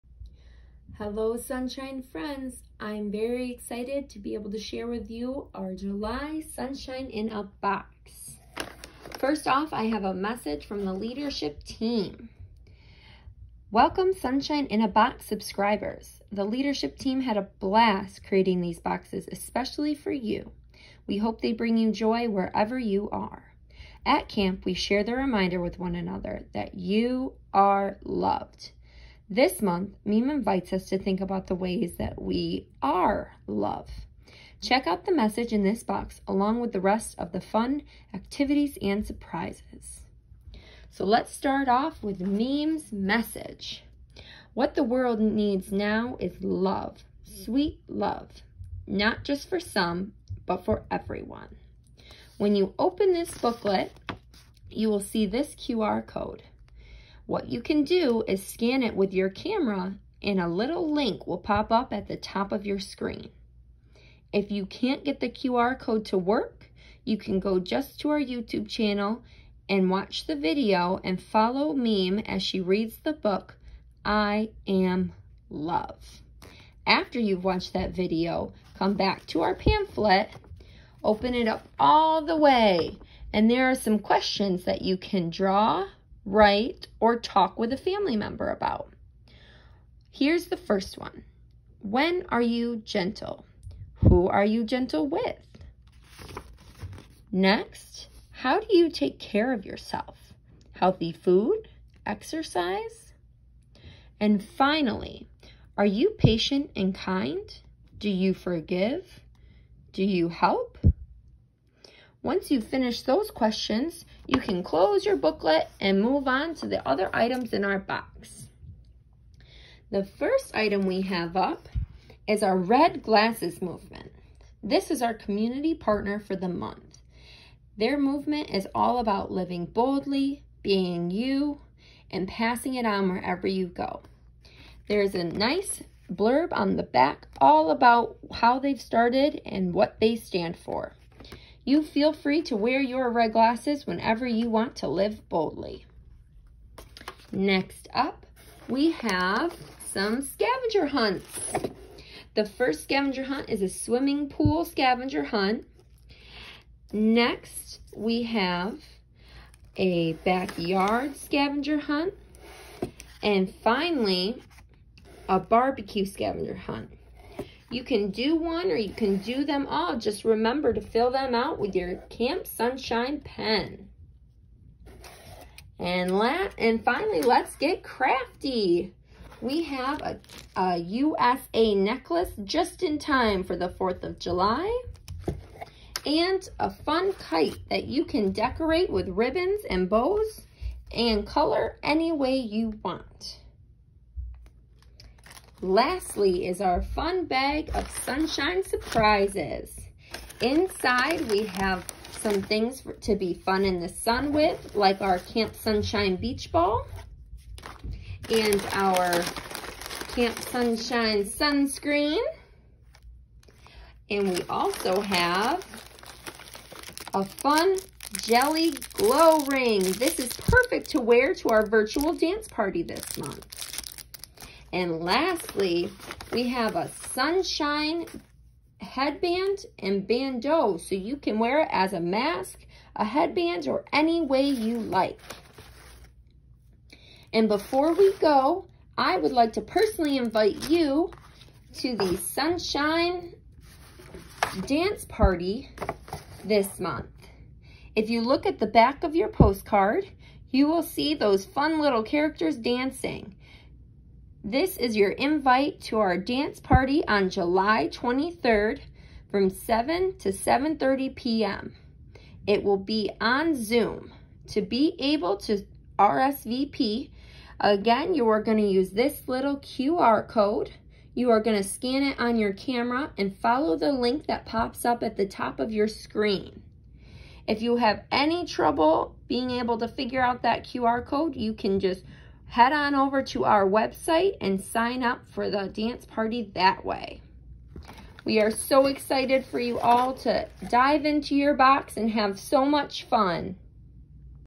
July Sunshine in a Box
Follow this unboxing video to find out what is inside your July Sunshine in a Box!